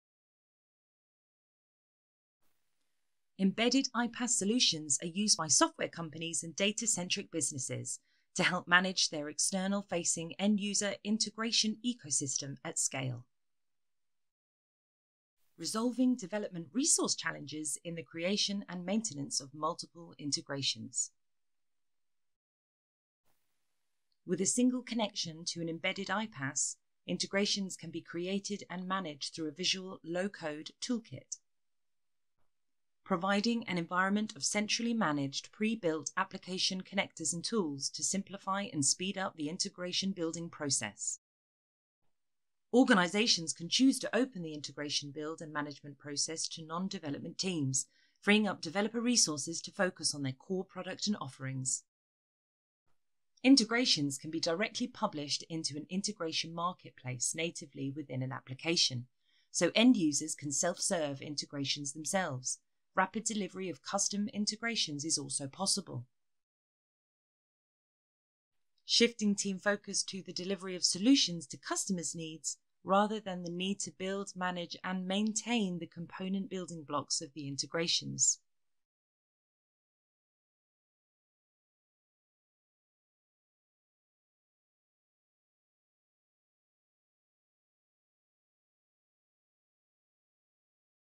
What is Embedded iPaaS?
Cyclr explains what Embedded iPaaS is, why they are a useful platform, and how they can be used to connect APIs, create integrations and manage your workflows.

Looking to trial an integration platform? Cyclr offers a free demo, just speak to one of our integration experts